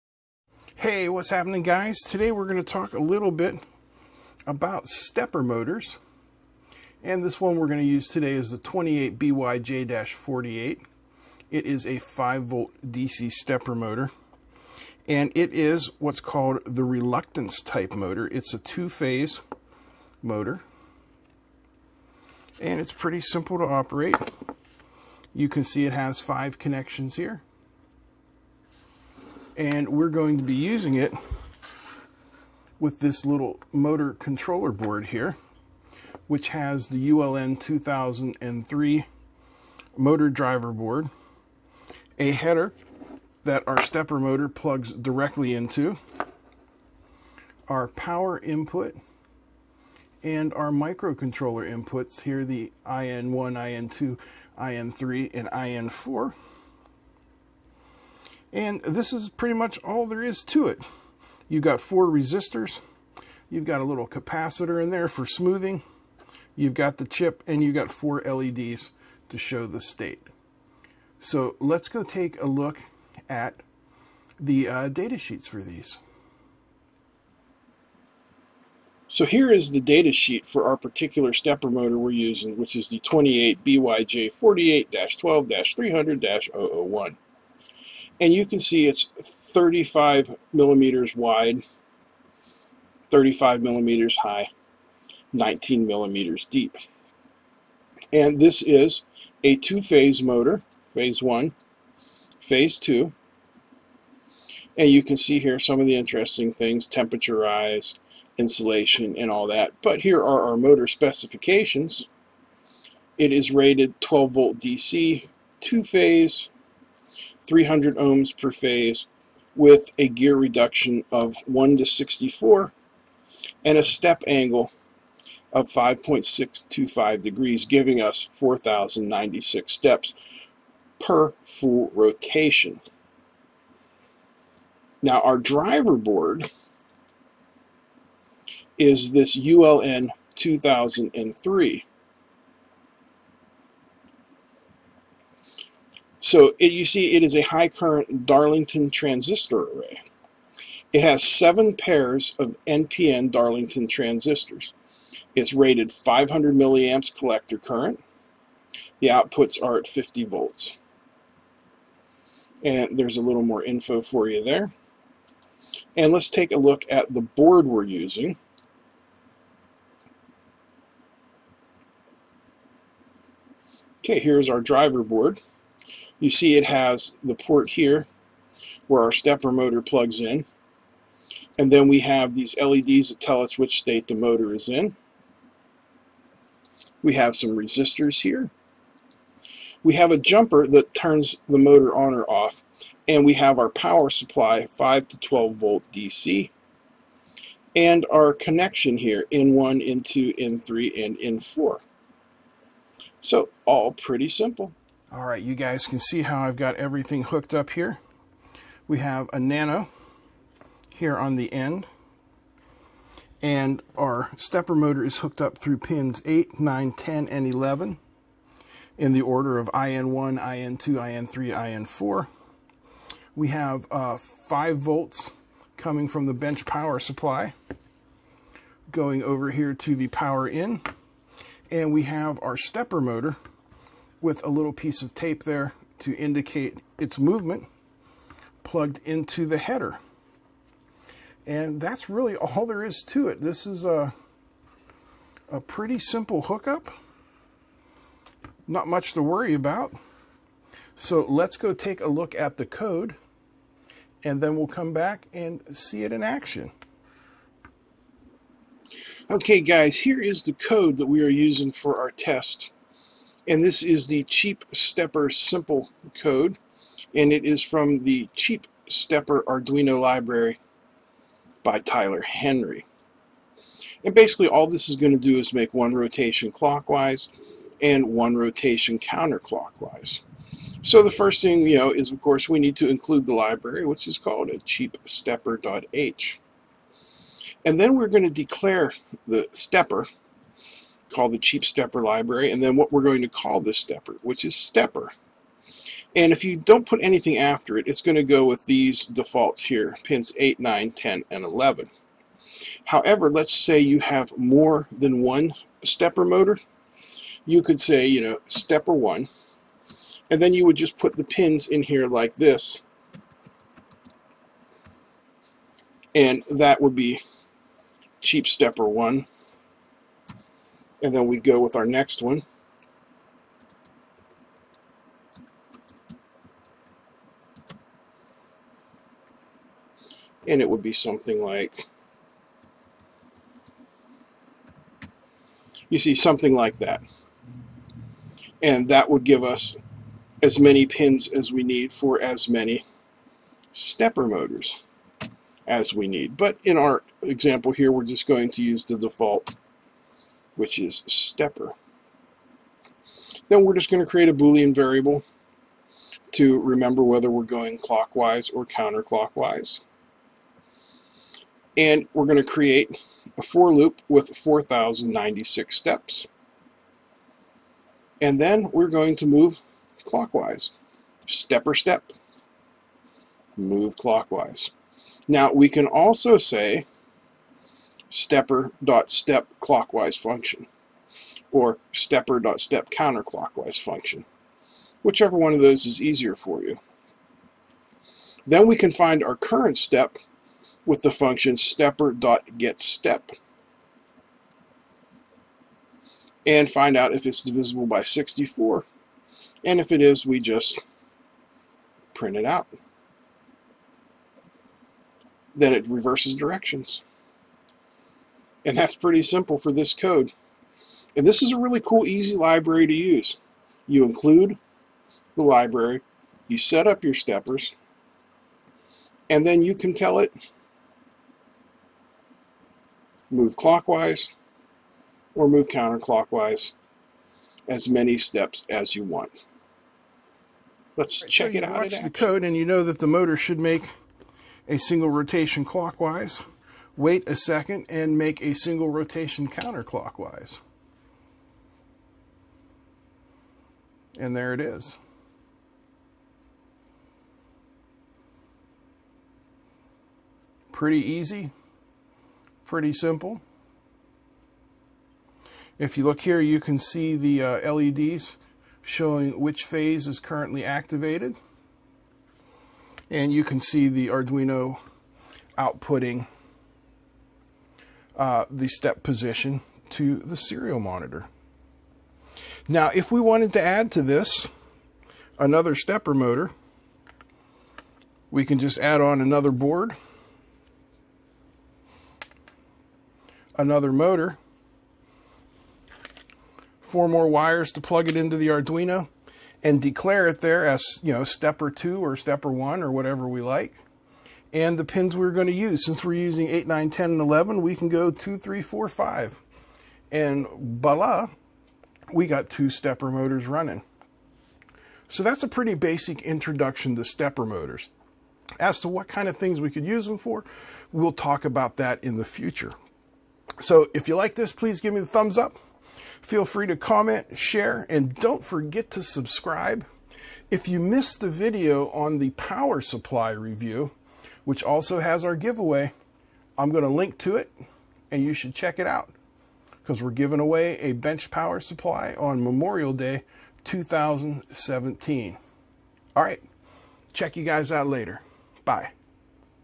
Arduino Basics Intro to Stepper Motors
In this video we take a look at very low-priced, but still very popular stepper motor, the 28BYJ-48. This 5V stepper is very useful in low-torque and medium torque applications where precise positioning is required.

We are using the ULN2003 driver IC to control the board along with the "Cheap Stepper" library.

Also, if you missed the Power Supply Review video eariler, here's a We are giving one of these away on Memorial Day 2017. The details to enter are in the video.